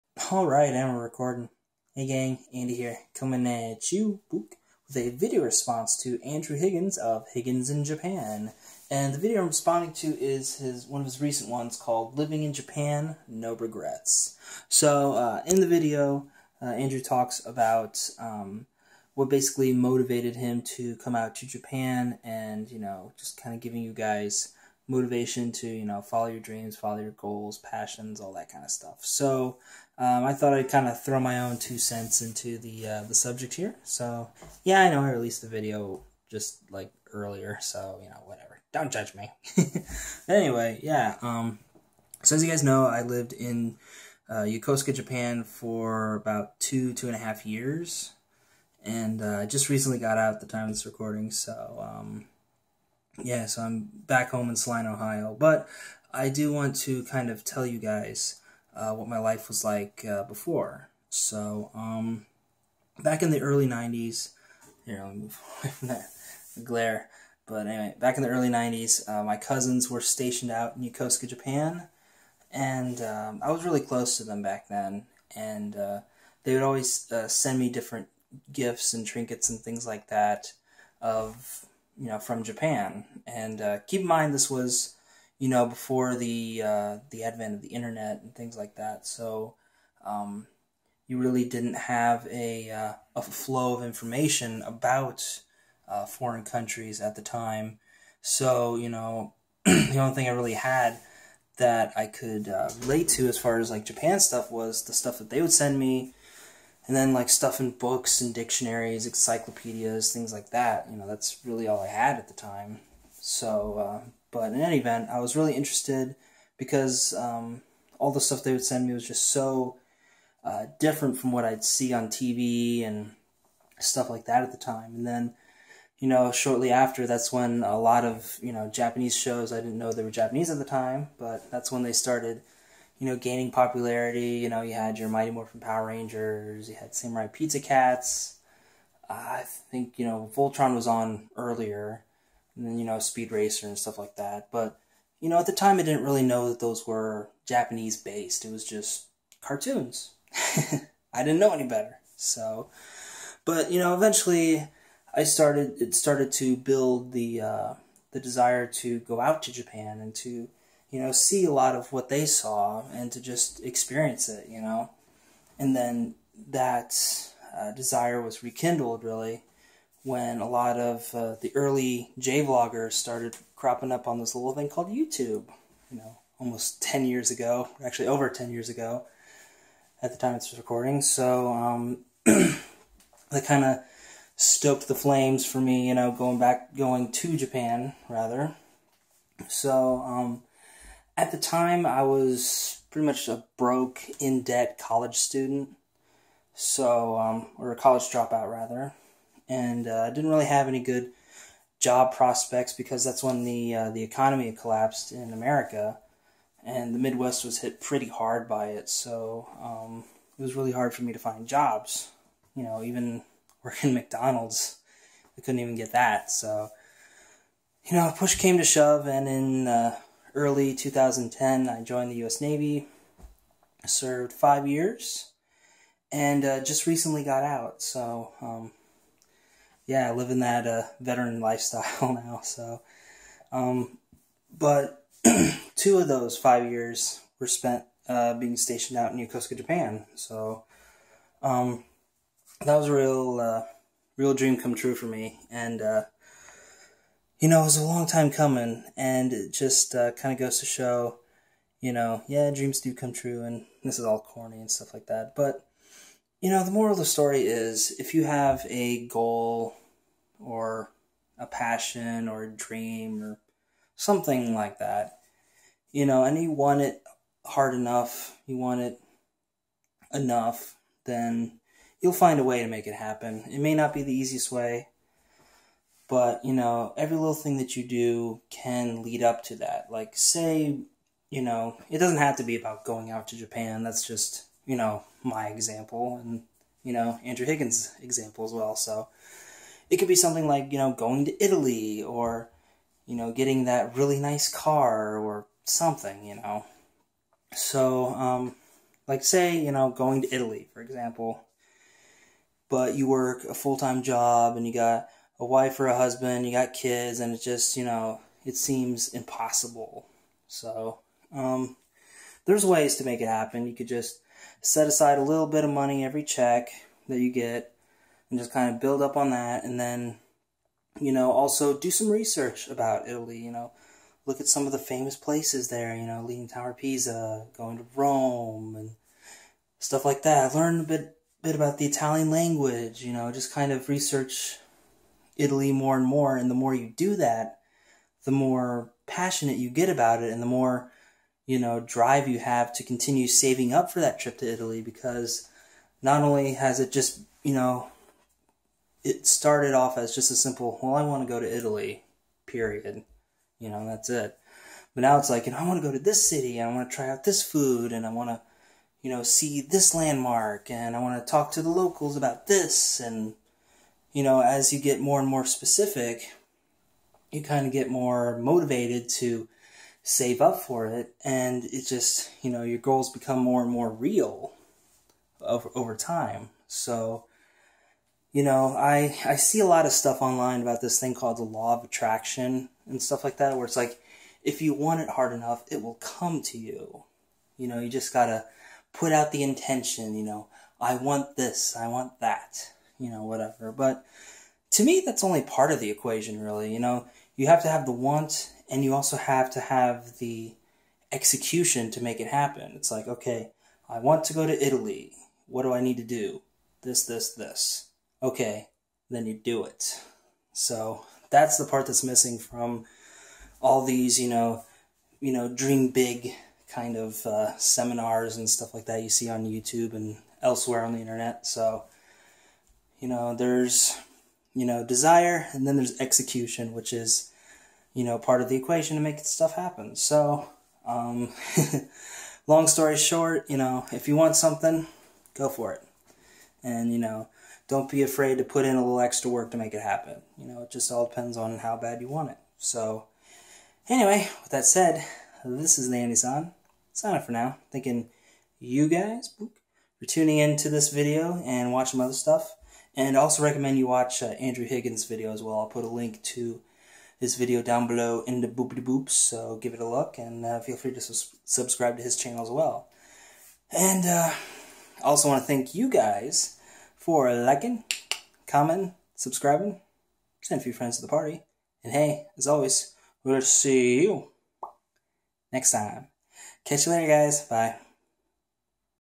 No Regrets Living In Japan - Vlog 212
Here's a video response to Andrew Higgins on following your dreams!!

TheAndySan

**Tech Notes**

*Camera Used*

LG G4